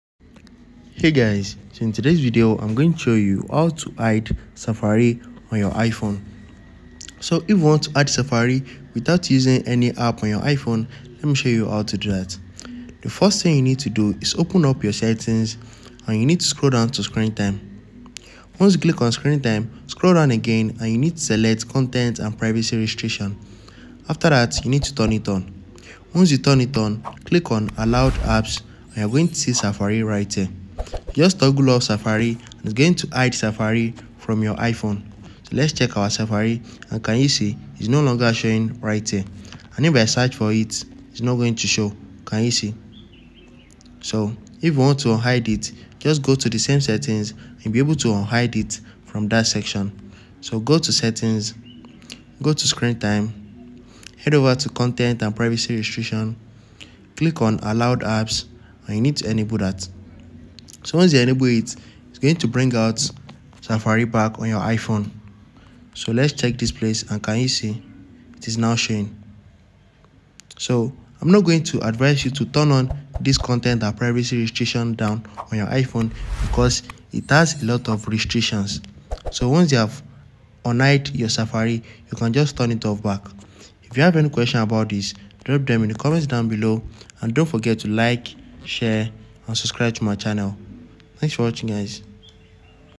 How to Hide Safari on iPhone! (2025) - Quick and Easy Guide!
Enhance your privacy with this quick guide! 🕵️‍♂️📱 Learn how to hide Safari on your iPhone and keep your browsing habits discreet. Watch now to master this essential privacy tip!